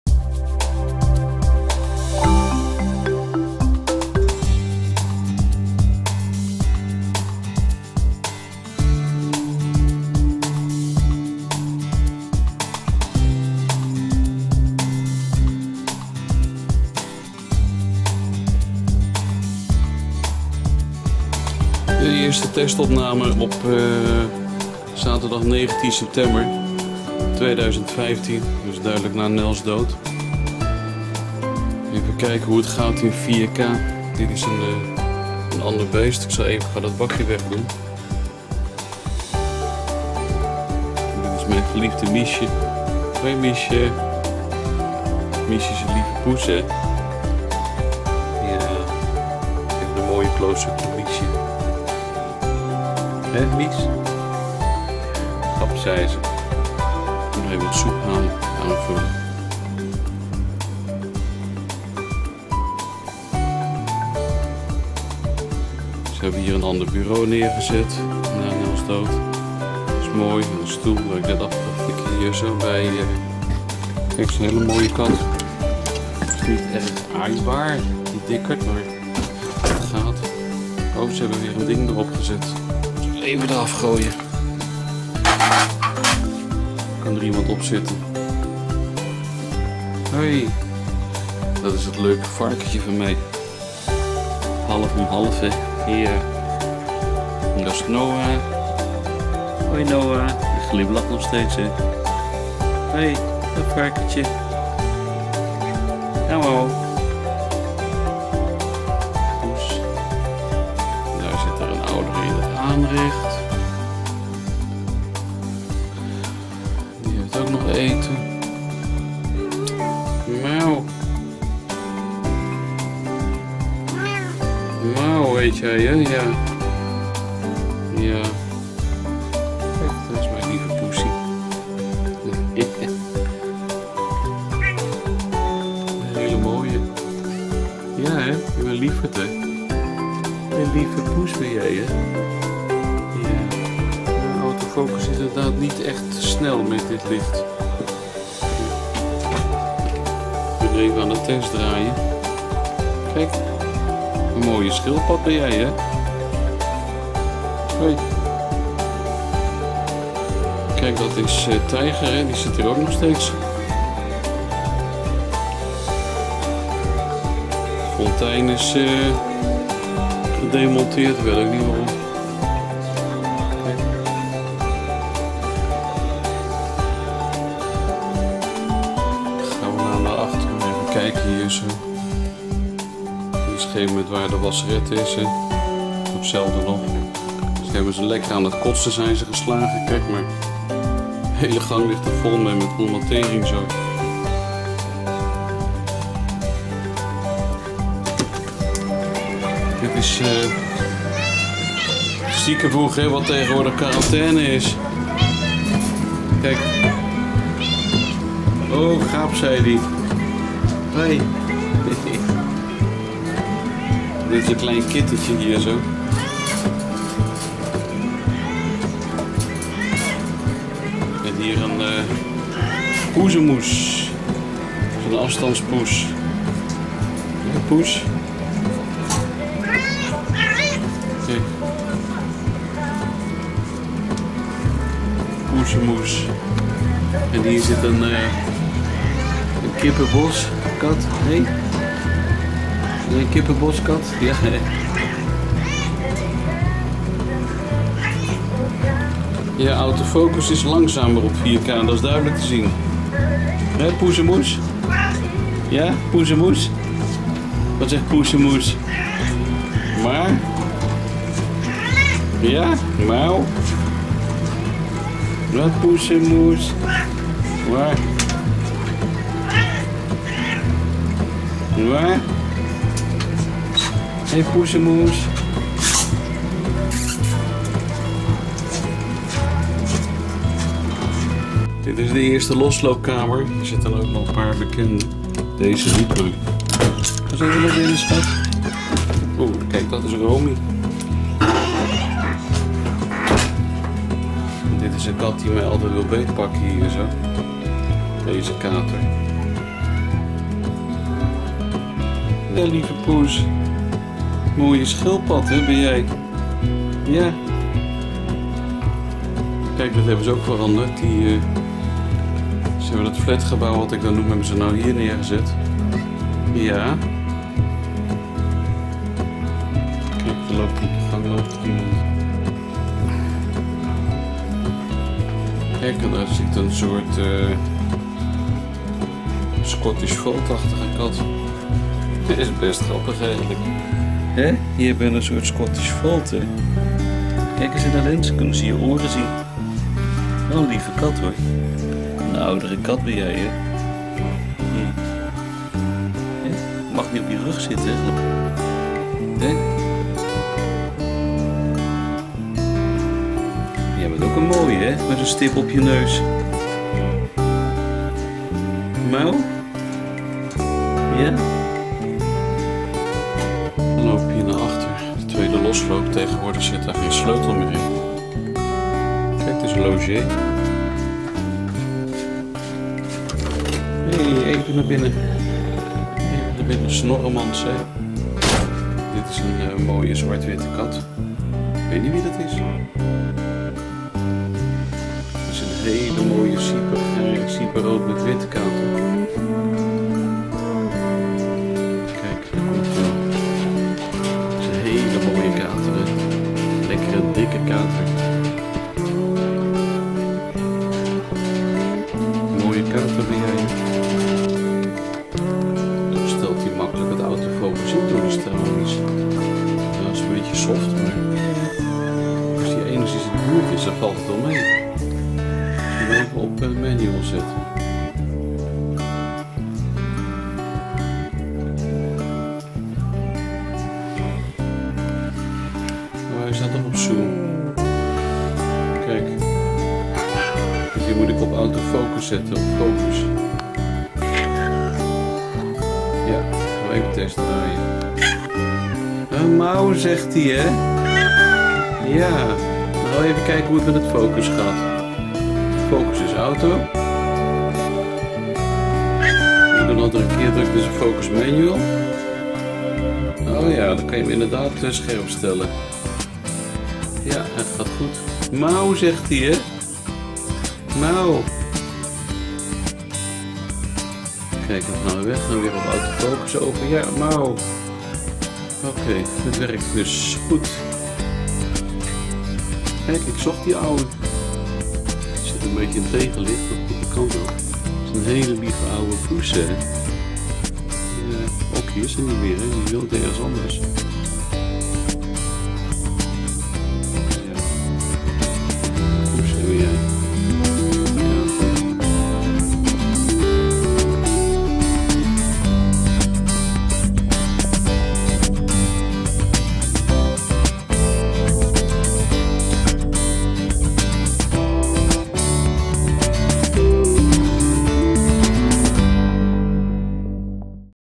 Kattenmand Asiel DierenLotDierenambulance test 4K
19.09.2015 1e Testopnamen op 4K in de Kattenmand met de Sony FDR-AX33 (available light, geen Rode mic, geen extra Videolamp. met UV-filter als lensbescherming) - produced: 4K 4095x2160/25p (50Mbps)